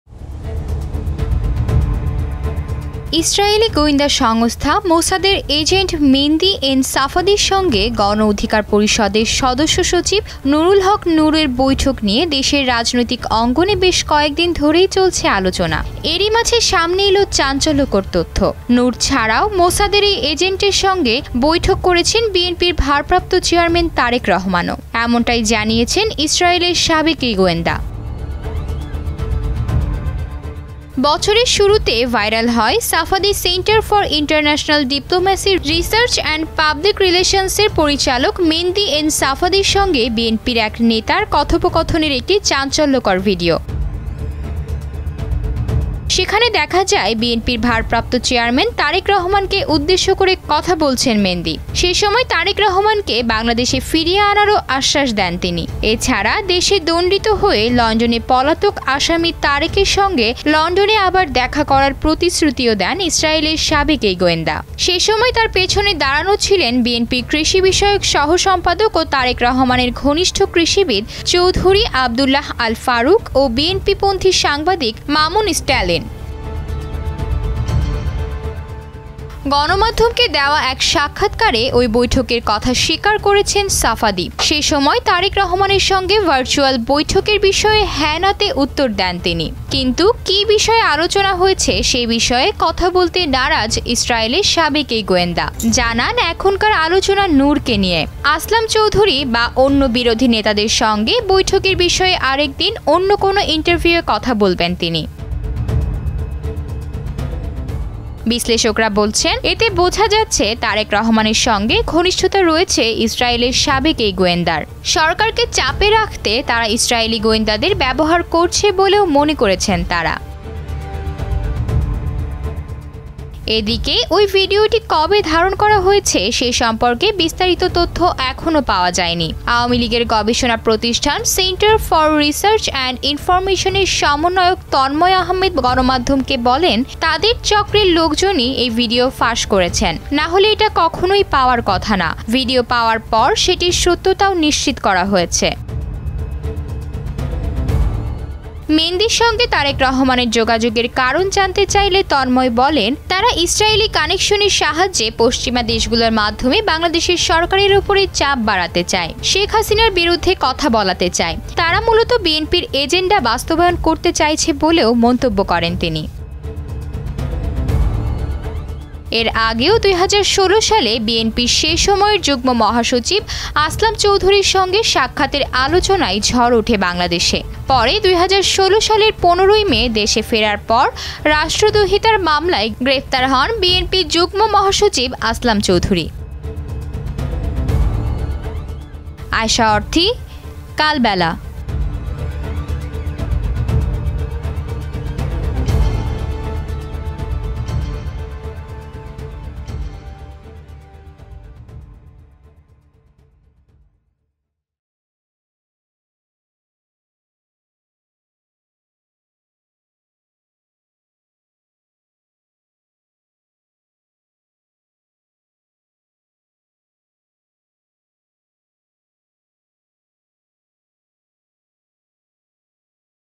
বিএনপির হাইকমান্ডের যে নেতার সঙ্গে বৈঠক হয়েছে, জানালেন সাফাদি | Mendi N Safadi | BNP | Kalbela
ইসরায়েলি গোয়েন্দা সংস্থা মোসাদের এজেন্ট মেন্দি এন সাফাদির সঙ্গে গণ অধিকার পরিষদের সদস্যসচিব নুরুল হক নুরের বৈঠক নিয়ে দেশের রাজনৈতিক অঙ্গণে বেশ কয়েকদিন ধরেই চলছে আলোচনা। এরই মাঝে সামনে এলো চাঞ্চল্যকর তথ্য, নুর ছাড়াও মোসাদের এই এজেন্টের সাথে বৈঠক করেছেন বিএনপির ভারপ্রাপ্ত চেয়ারম্যান তারেক রহমানও। এমনটাই জানিয়েছেন ইসরায়েলের সাবেক এই গোয়েন্দা। বিএনপির হাইকমান্ডের যে নেতার সঙ্গে বৈঠক হয়েছে, জানালেন সাফাদি | Mendi N Safadi | BNP | Kalbela